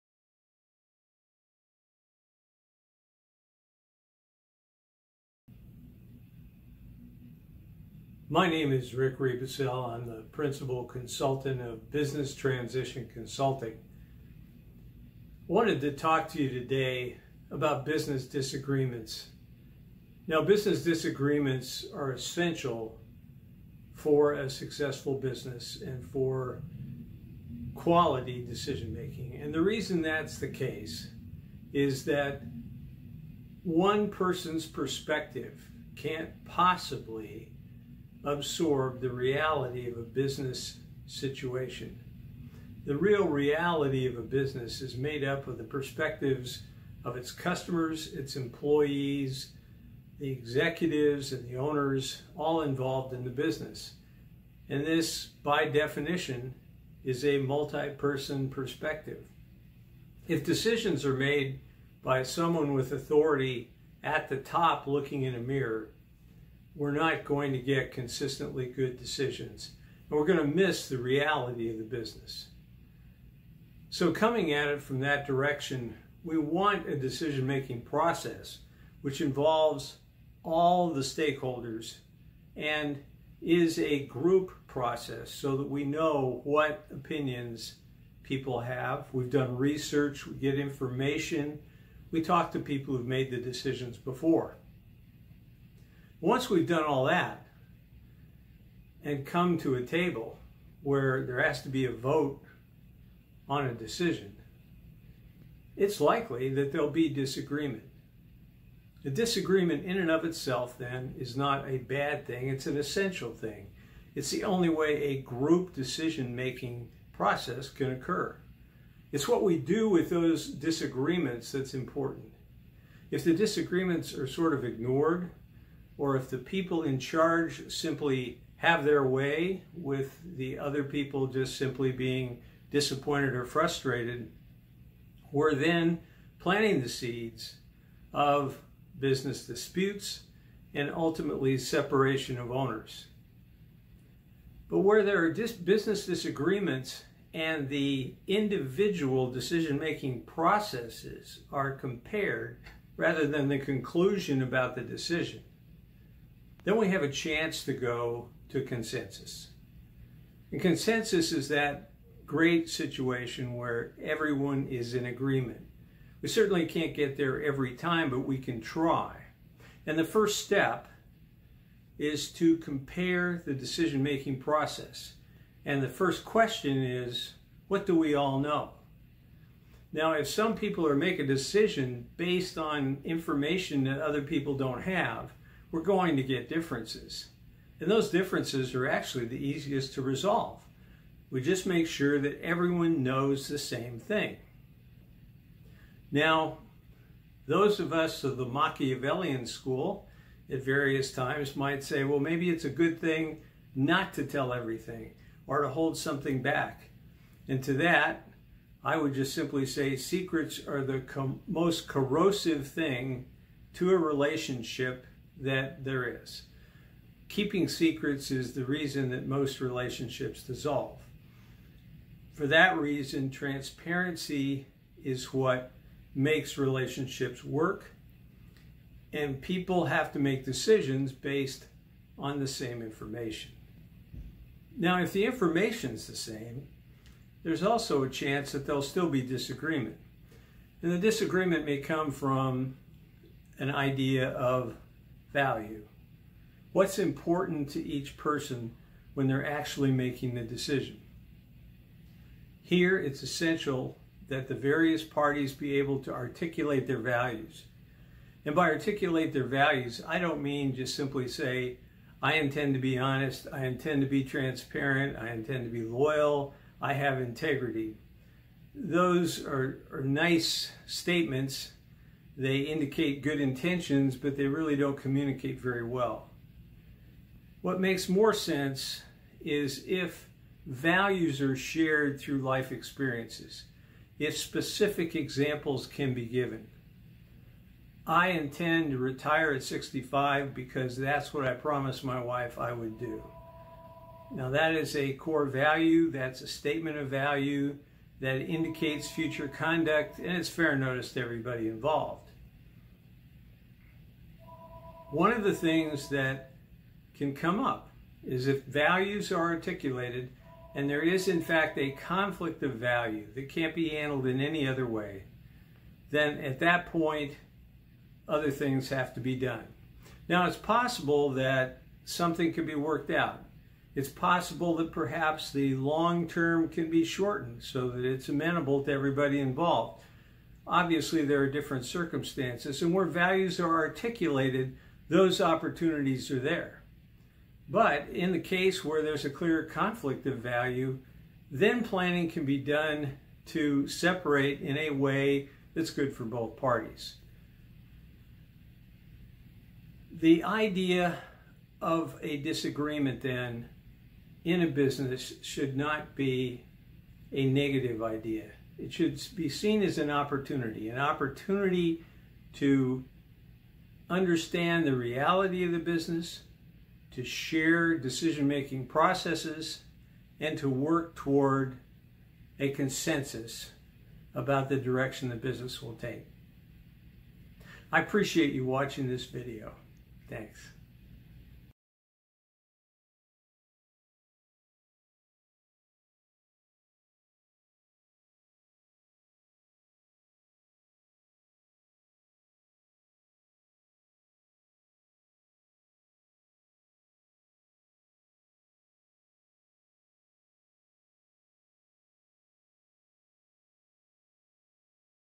Business Disagreements
Suppressing even minor disagreements now will lead to disputes later on. Dealing with disagreements as an opportunity will provide for accomplishing consensus and allow better business decisions to be made.